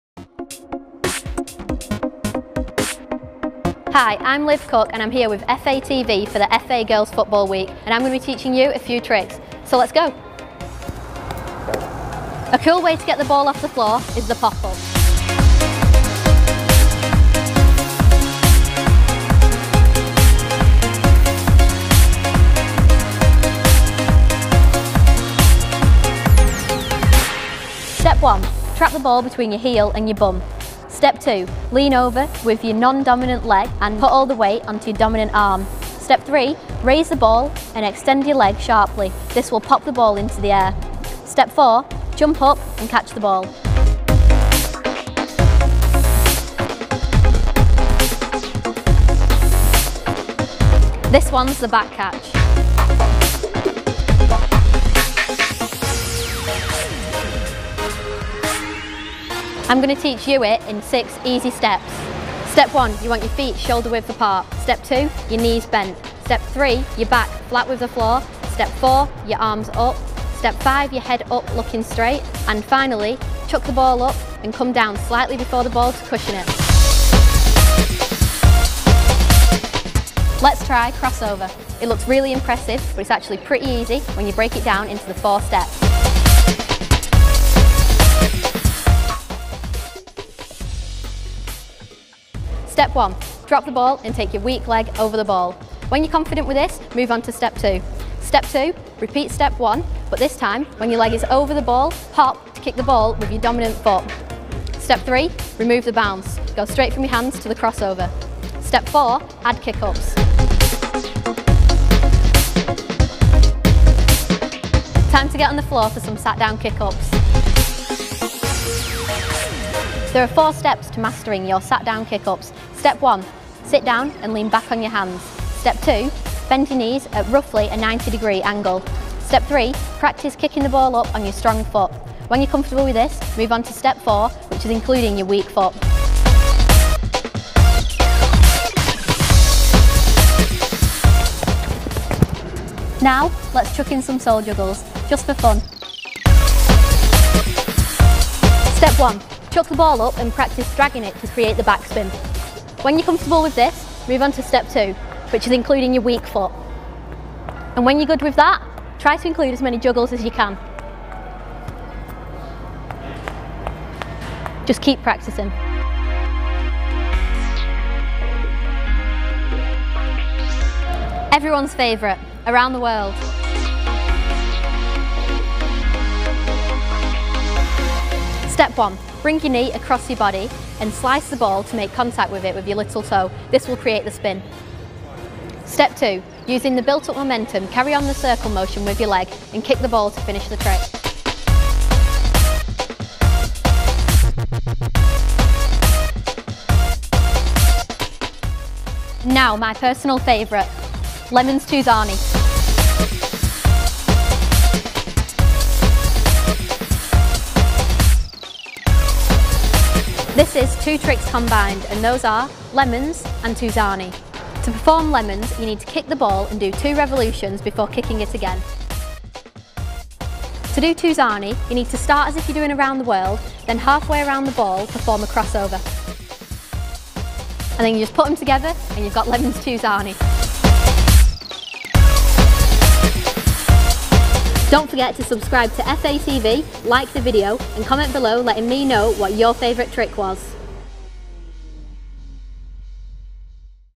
Learn 7 Football Freestyle Tricks with Liv Cooke | Masterclass
Britain's first professional female freestyler, 16-year-old Liv Cooke, joined FATV for Girls Football Week, teaching those at home some basic freestyle moves to get you started.

What is Girls Football Week?
Girls Football Week is aimed at increasing participation in girls football between the ages of 5-16.